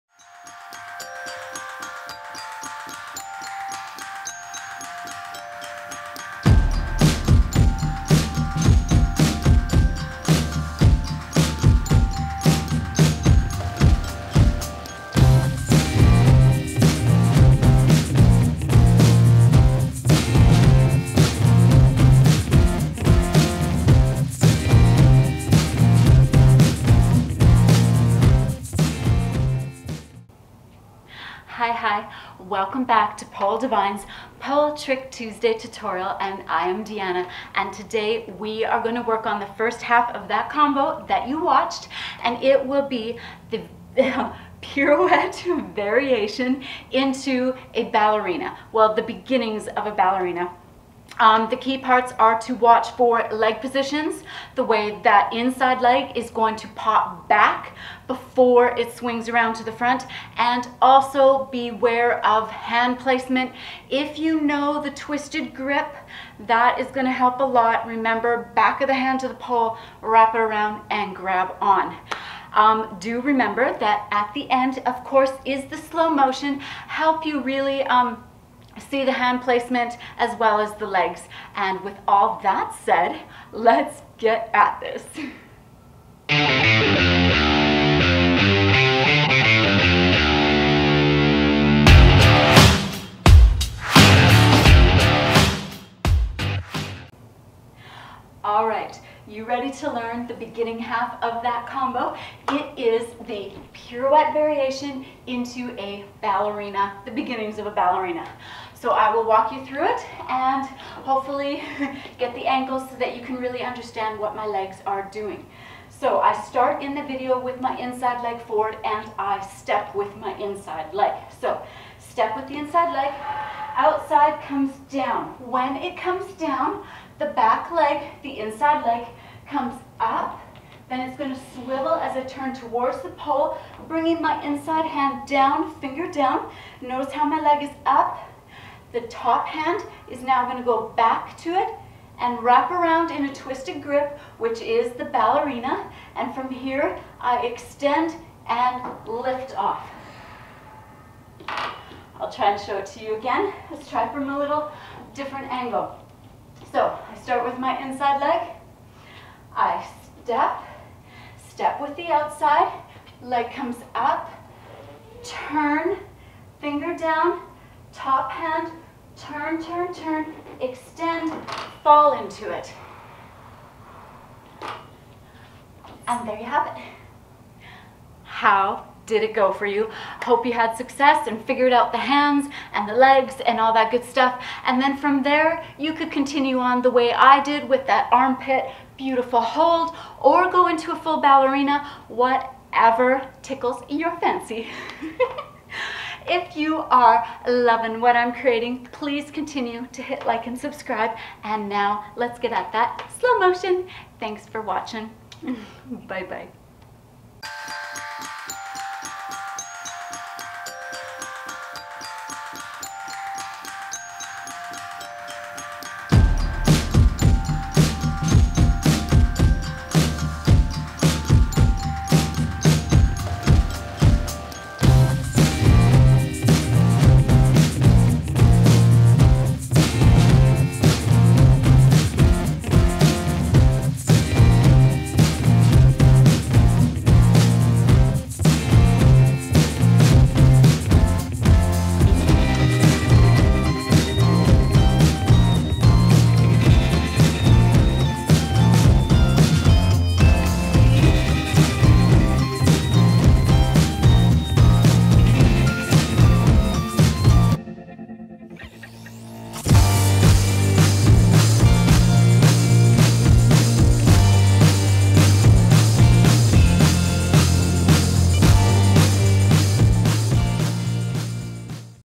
Pole Trick Tutorial - Pirouette to Ballerina combo (master pole fitness)
HiHi

Pole Trick Tutorials are a fun fast breakdown of favourite tricks or transitions to add to your bag of pole moves and pretty shapes!  The beginning is a pole combo performed by Deanna, which has the trick or transition tucked strategically within.  She then gives the intro of what we will be learning, followed with the tutorial.  In ending, Deanna shows you the beginning combo in slow motion so that you can visually see the trick or transition as well as the rest of the combo, if you are wanting to pick it apart and figure it out in its entirety.

Hope you enjoy this week’s Pole DeeVine Trick Tuesday Tutorial!

Sweet tricks,
Deanna